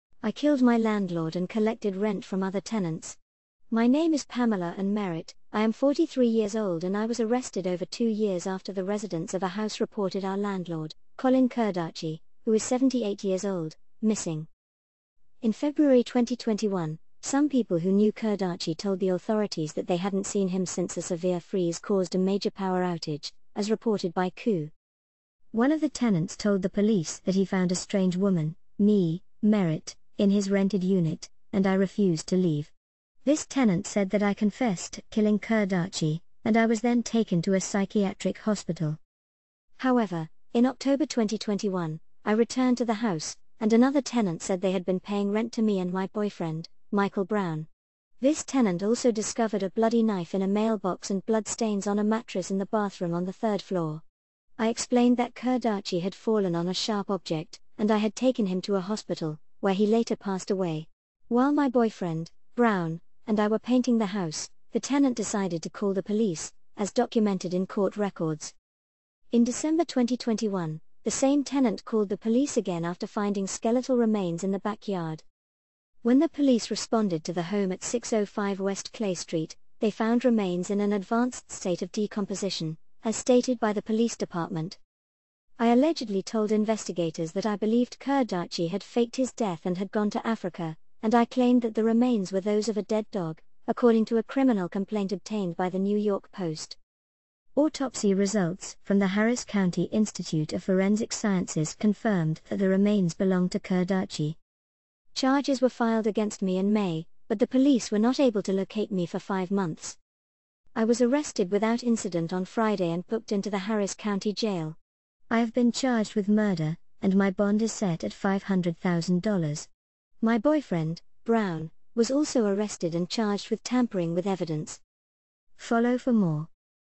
Pamela Ann Merritt allegedly killed her landlord, hid his body and collected rent from other tenants
Pamela Ann Merritt allegedly killed her landlord, hid his body and rented out his home and collected rent from other tenants.